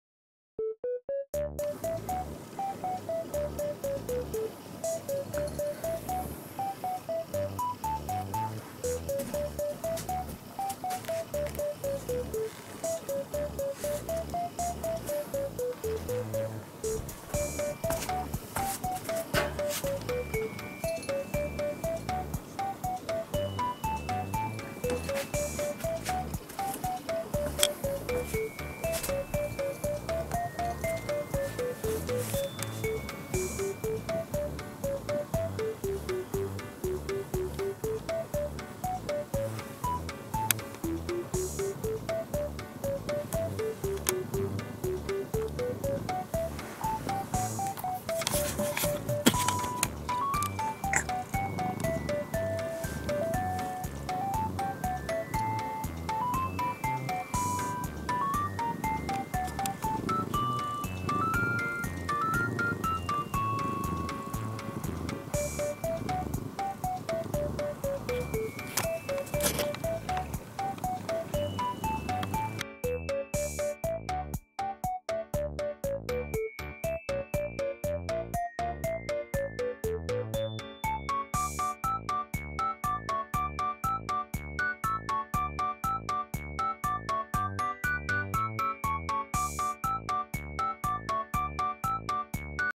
腹が減って擦り寄ってくる猫さん【侵入した野良仔猫】～家猫修行中～
目的のためなら手段をえらばない・・